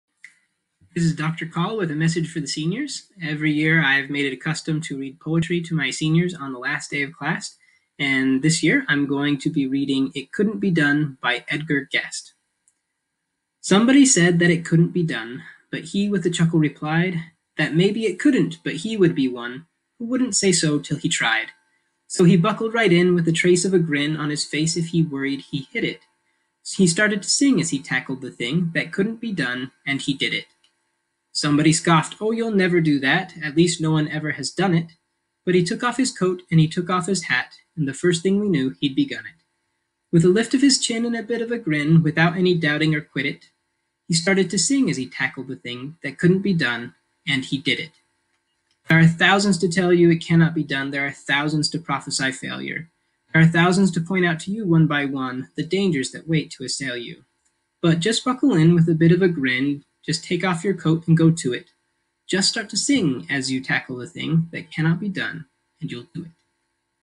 Physics Teacher Dr. Robert Call
Physics teacher Dr. Robert Call has a tradition of reading his seniors poetry on the last day of class. So, on this final day of school for the Class of 2020, enjoy as he reads "It Couldn't Be Done" by Edgar Guest.